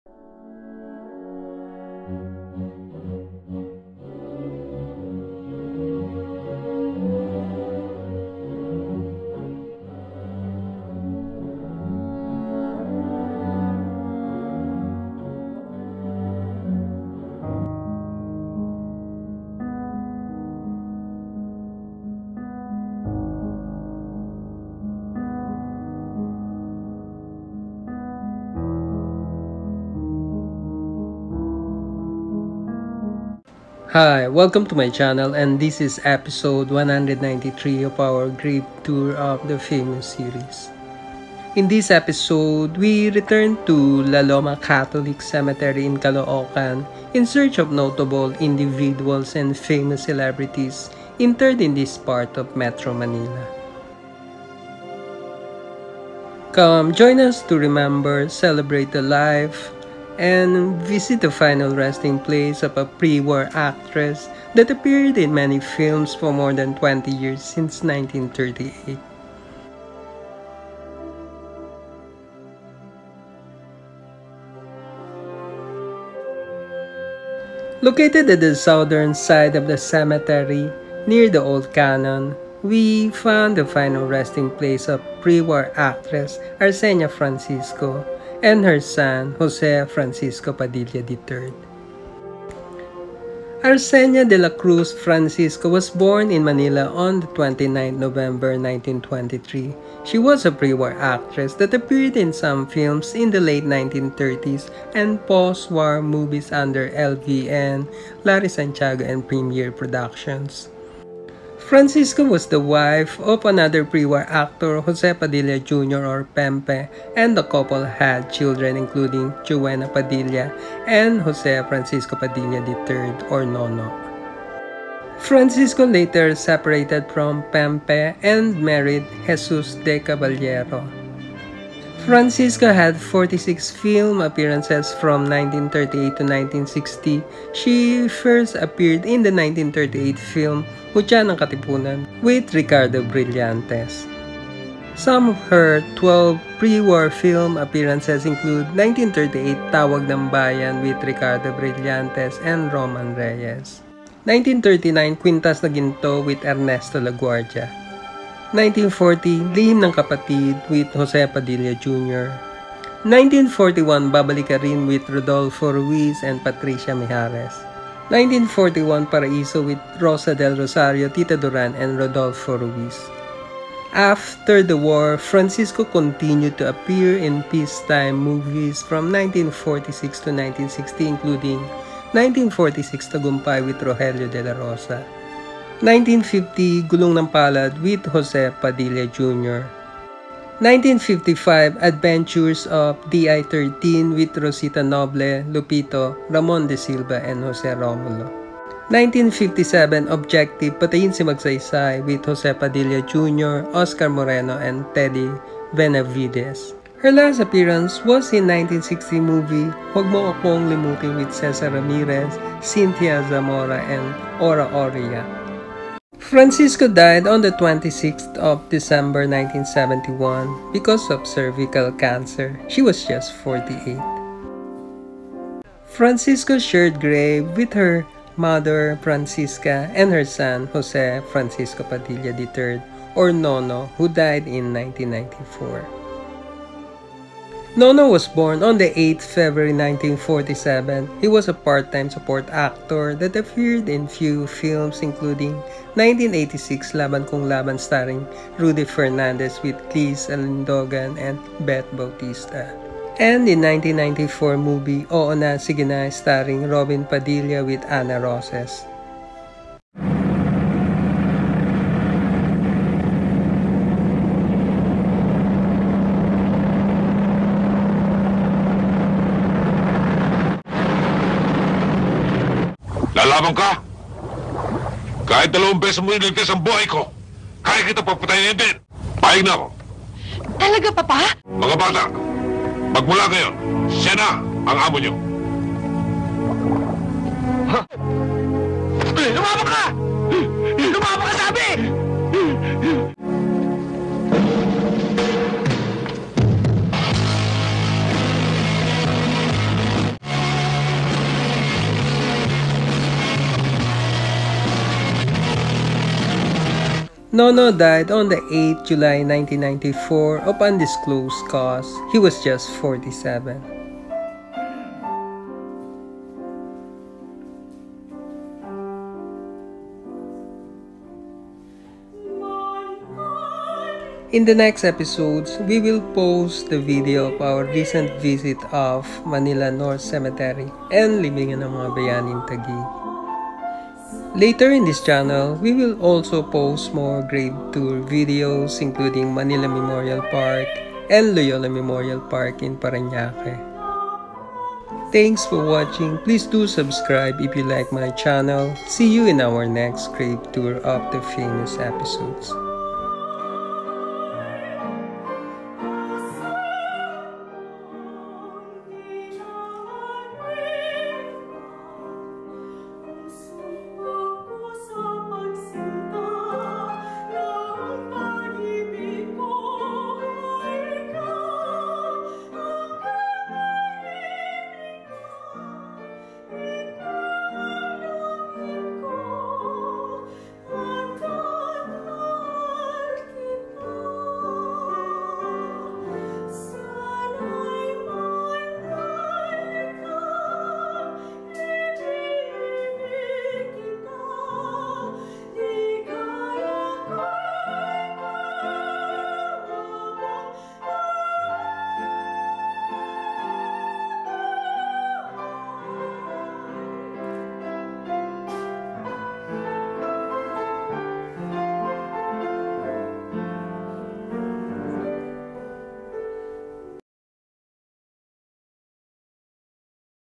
Gravetour of the Famous E193en | Arsenia Francisco | La Loma Catholic Cemetery -Caloocan
TAGALOG subtitle -Please activate Subtitle by pressing CC button on screen
Special thanks to ron tap for making this episode possible!

In this video we visited the final resting place of ArseniaFrancisco and her son JosePadillaIII at LaLomaCatholicCemetery in Caloocan.